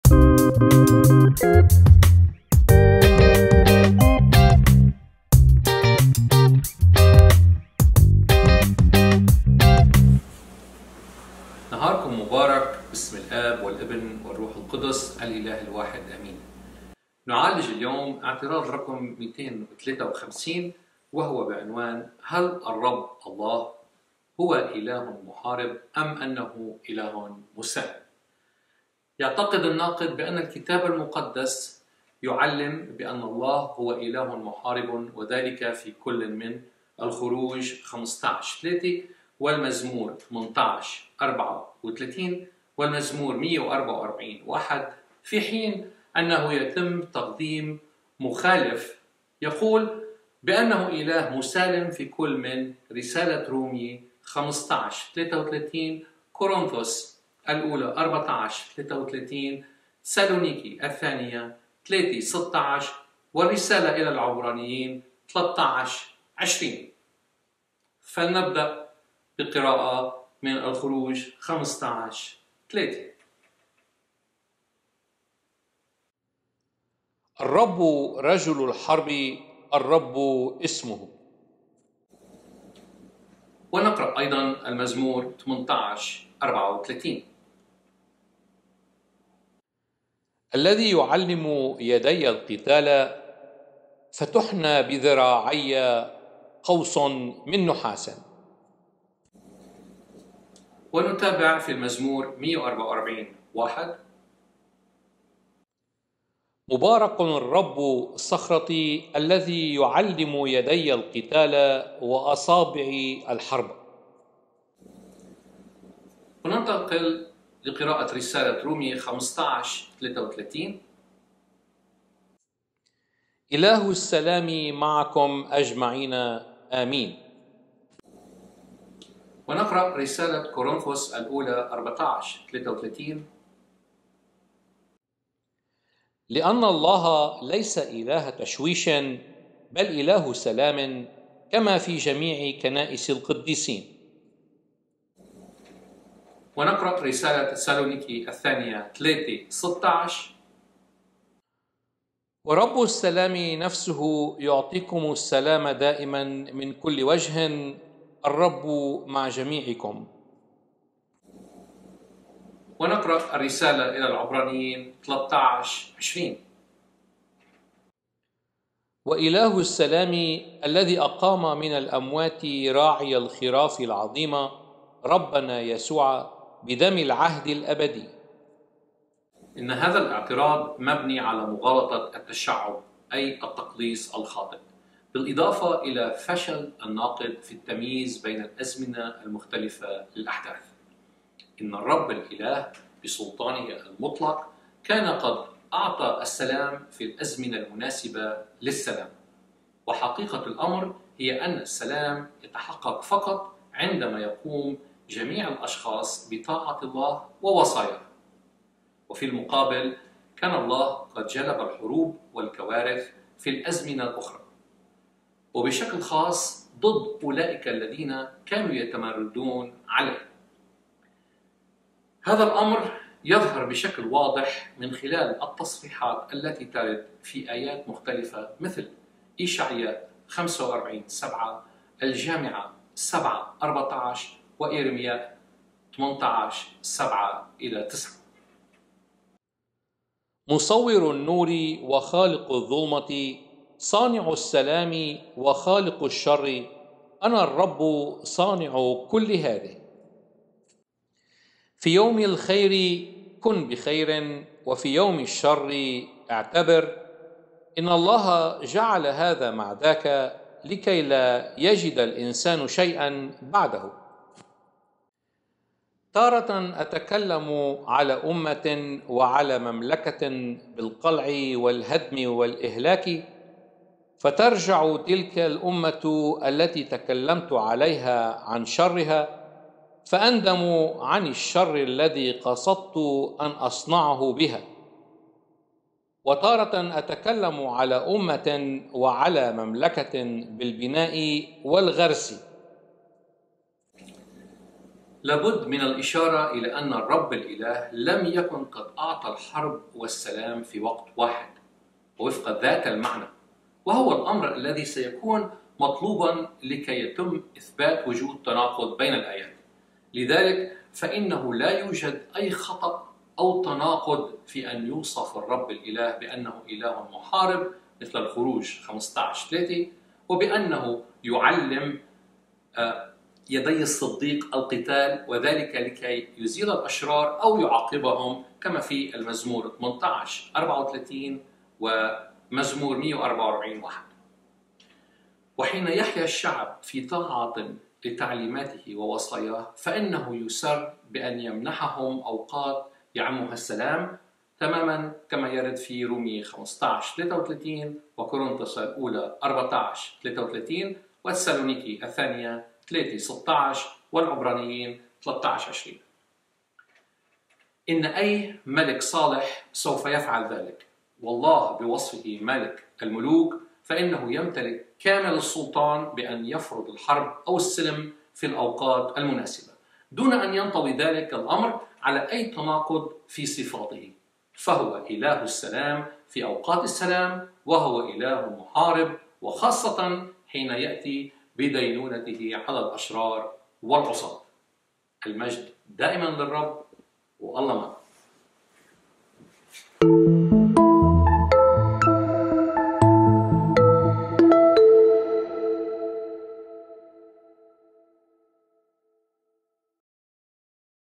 ٢٥٣ هل الله هو إله مُحارب أم أنَّه إله مُسالم؟
ما هو موقف الله من الحرب والسلم؟ يتساءل الناقد معتقداً بوجود تناقض بين تعاليم الكتاب المقدس، ولكن الإجابة ليست بعيدة عن ناظريه.
لمراجعة المزيد من المواضيع الرجاء زيارة موقعنا الإلكتروني من خلال الضغط على الرابط التالي: